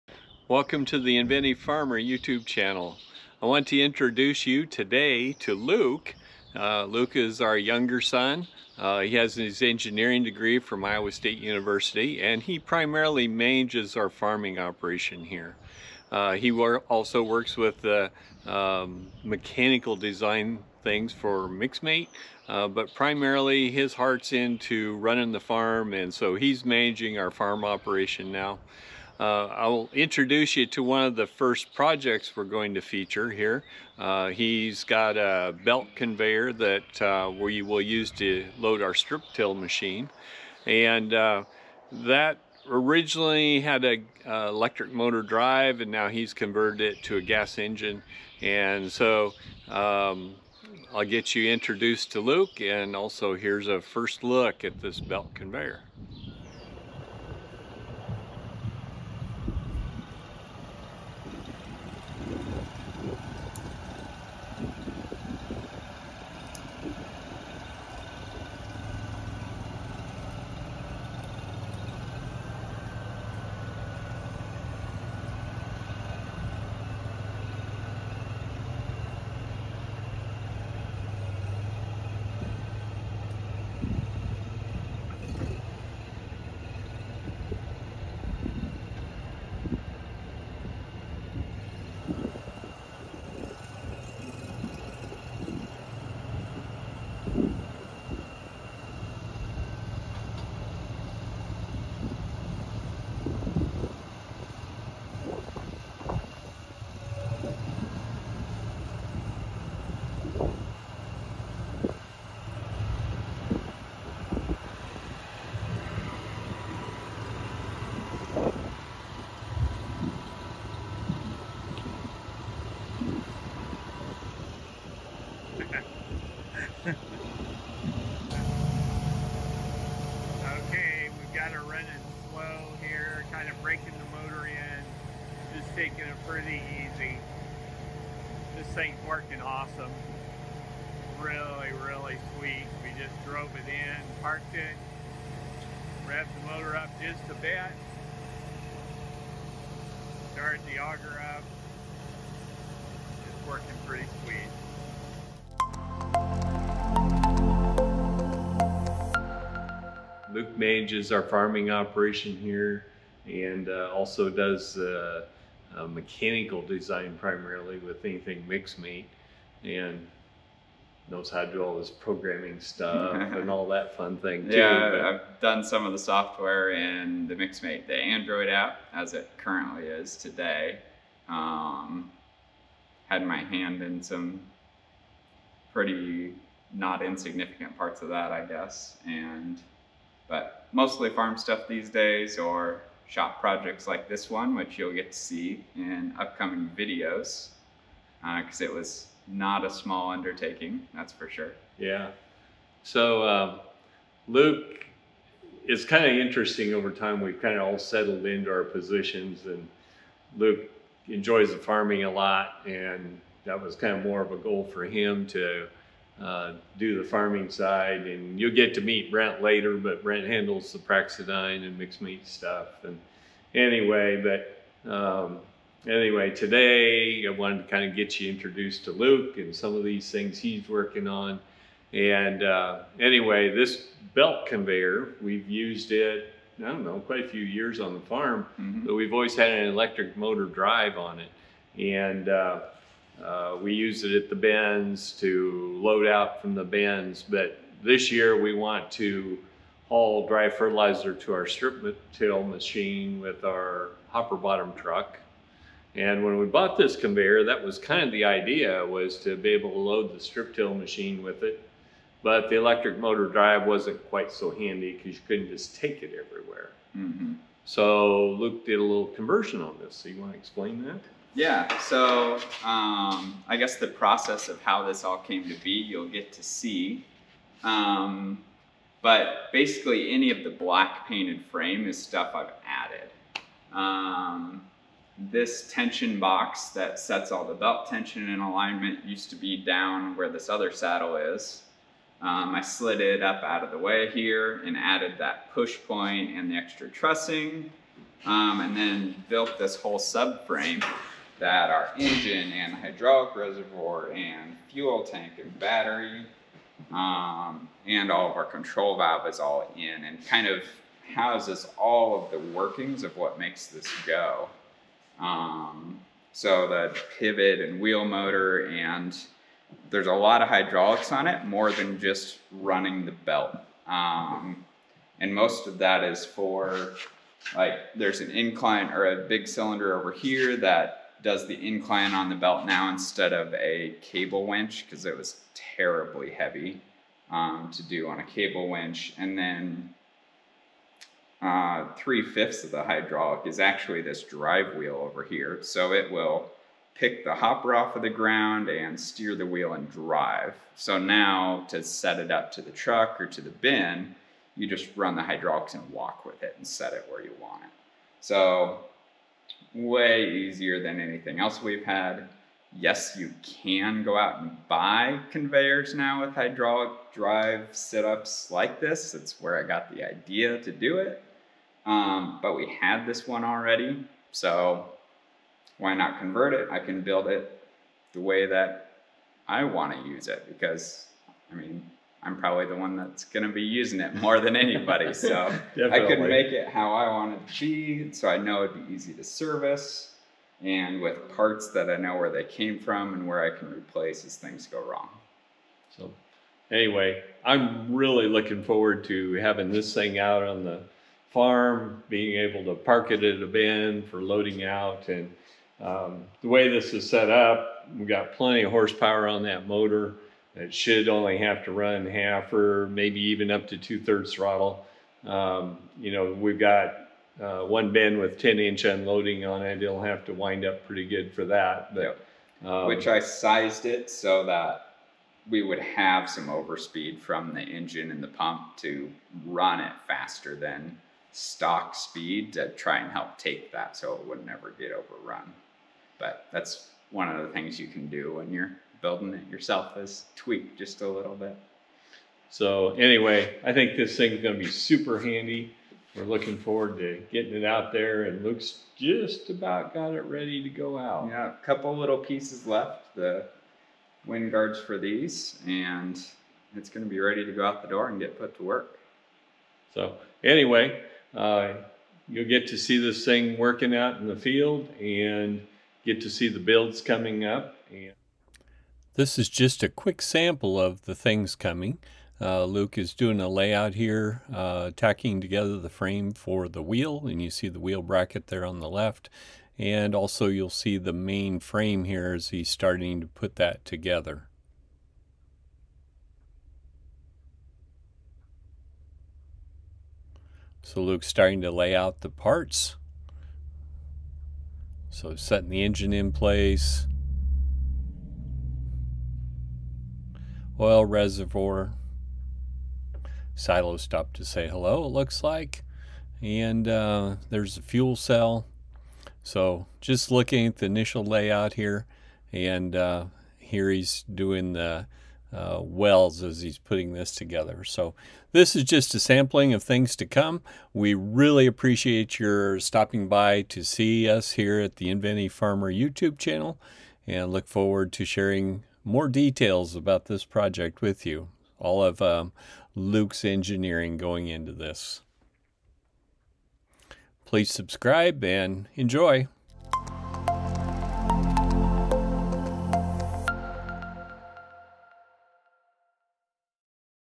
Introducing Luke and the Conveyor Project
Meet Luke and get a first look at the belt conveyor project where he converts our belt conveyor from an electric drive to a gas engine drive with a hydraulic-driven wheel and lift.  The conveyor is self-propelled so it is easy to move and set up.